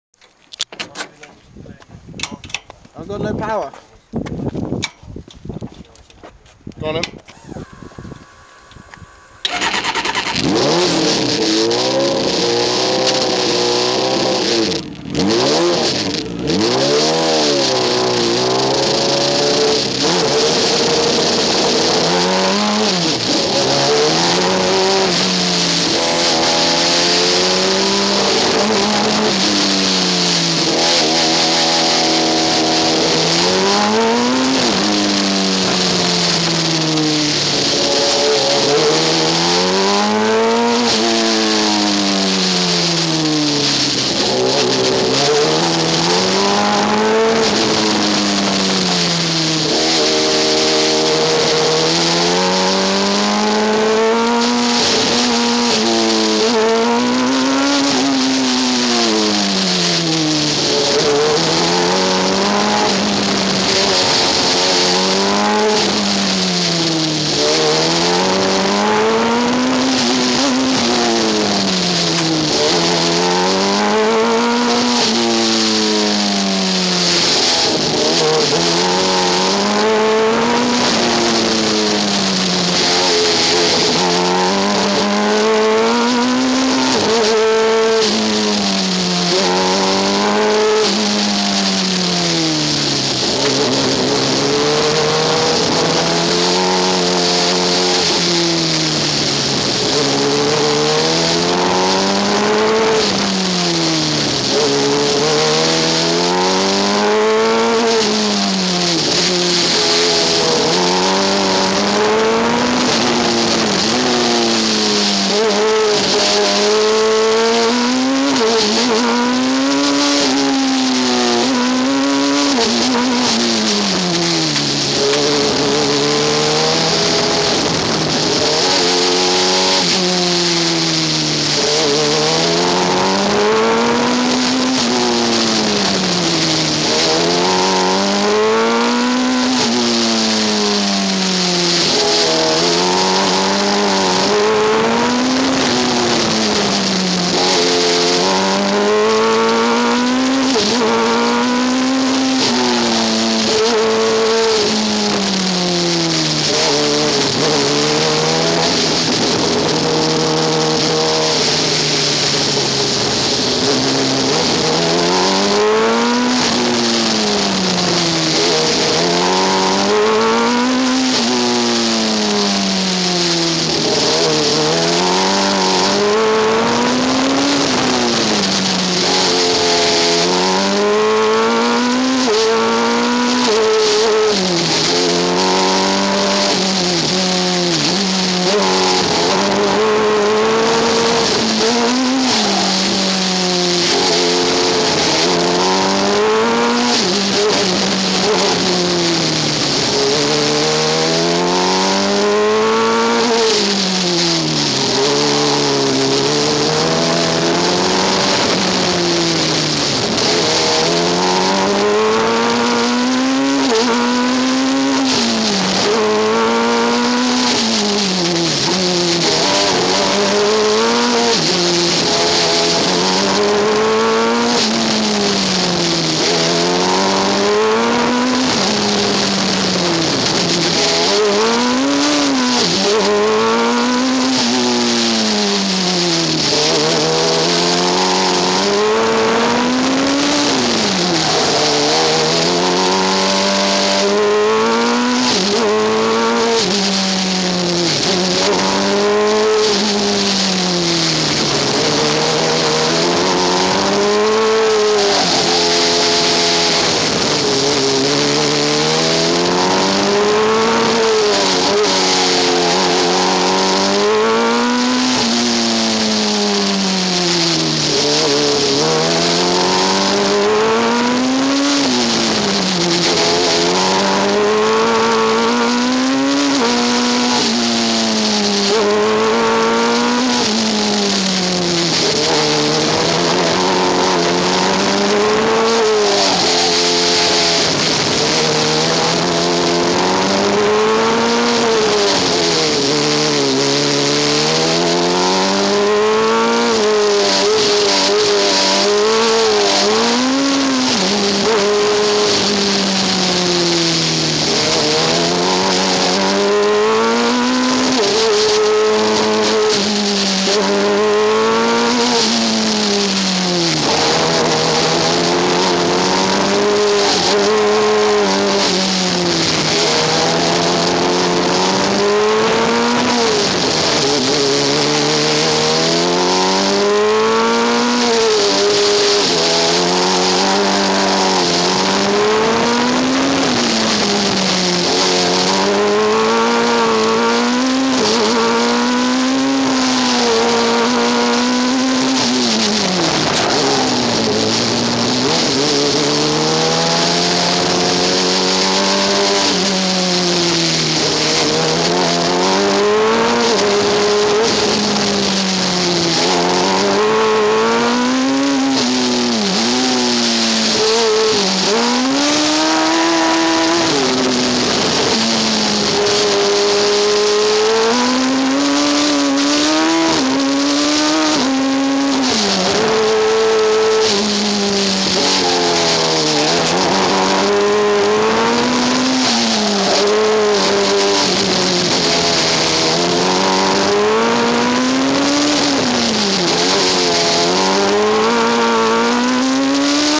Formula Student Coventry 2009
2009 class 100 Coventry University Formula student car testing at Bruntingthorp, Ryan Colvey driving